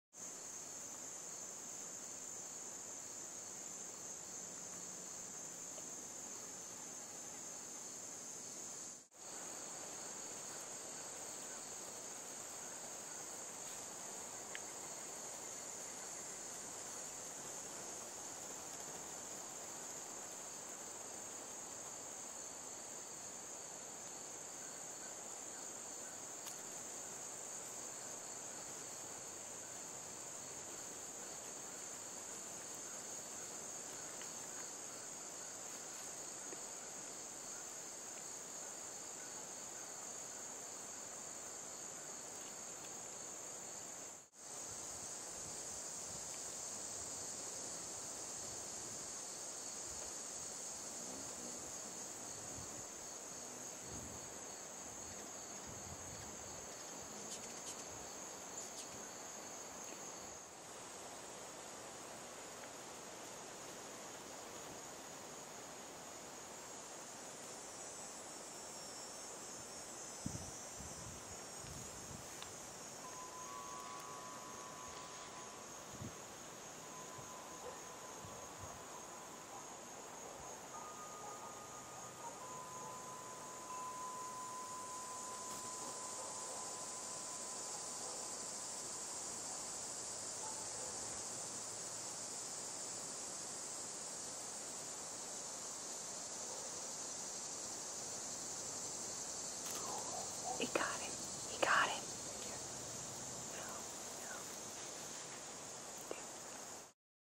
Praying Mantis captures and eats two Skipper Butterflies
Skippers are an easy meal for a Chinese Praying Mantis hunting among the Zinnia in my garden.